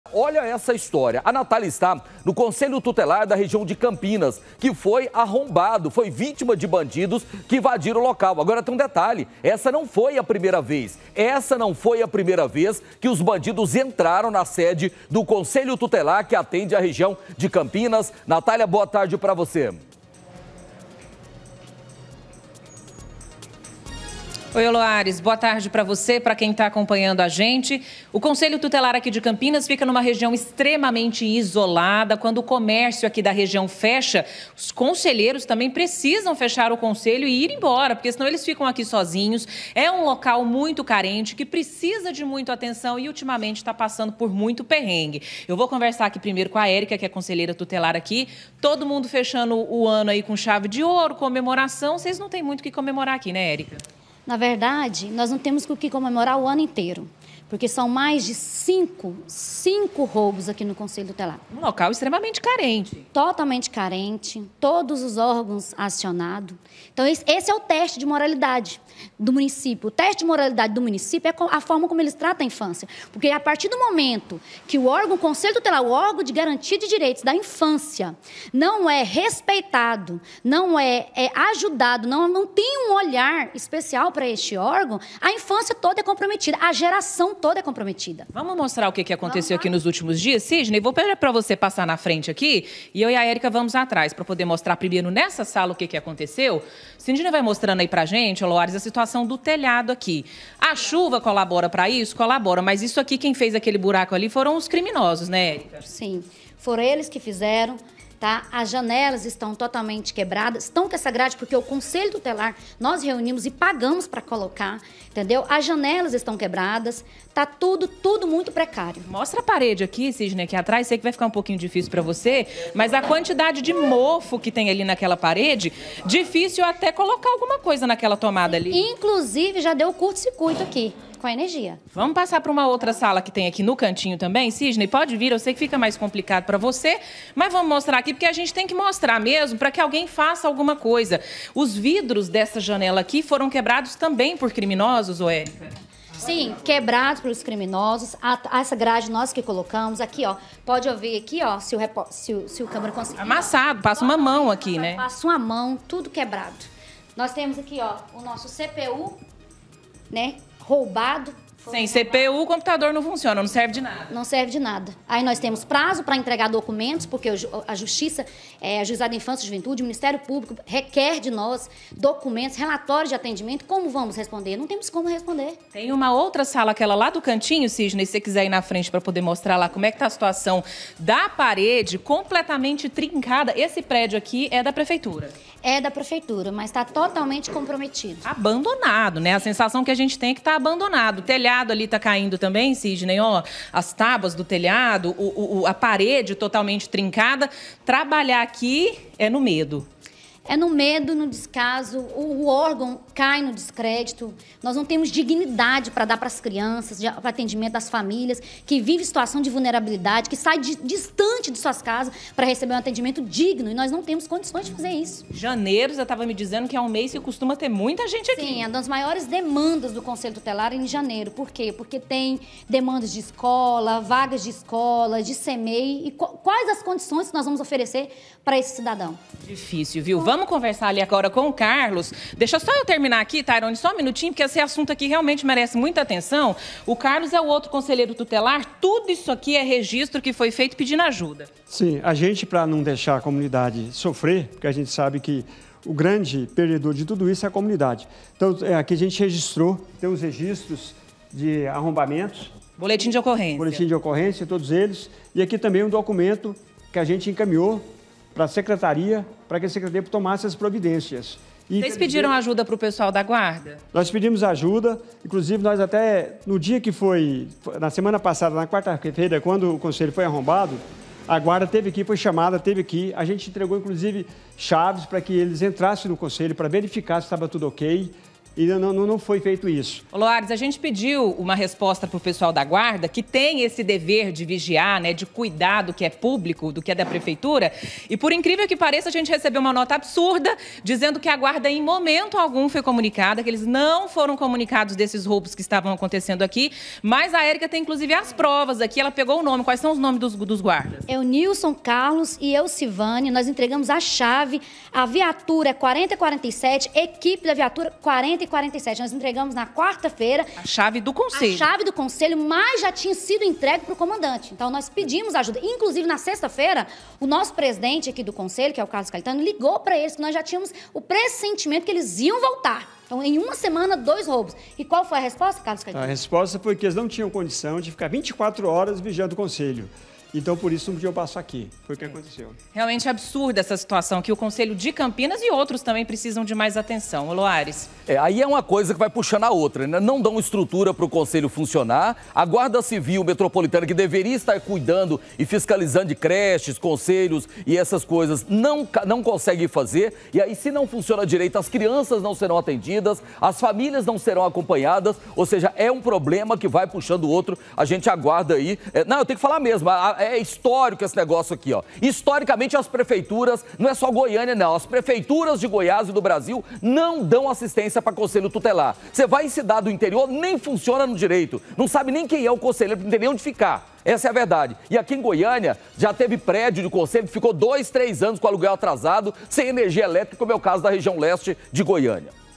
BANDIDOS INVADEM CONSELHO TUTELAR PELA QUINTA VEZ
NATHÁLIA MENDONÇA ESTÁ NO CONSELHO TUTELAR DE CAMPINAS. JÁ É A QUINTA VEZ QUE OS BANDIDOS INVADEM O LOCAL.